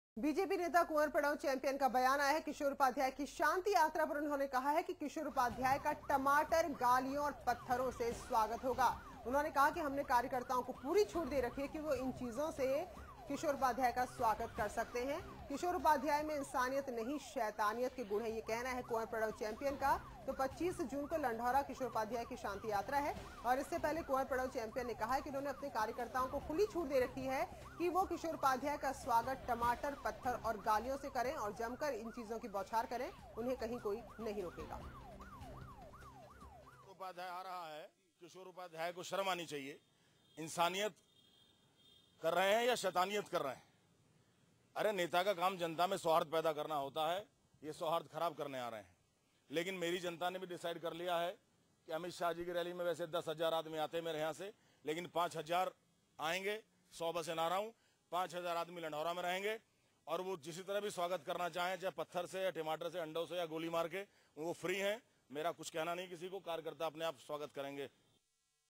Kunwar Pranav Singh Champion says about Kishor Upadhyay
kunwar pranav singh champion told about Shanti Yatra of Kishor Upadhyay.